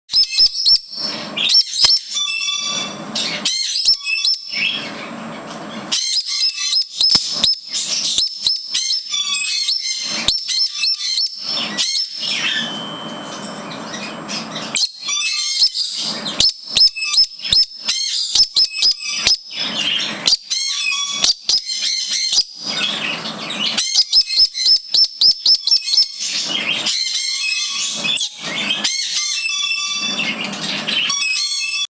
Dewey 4
Dewey is glad that I am home from work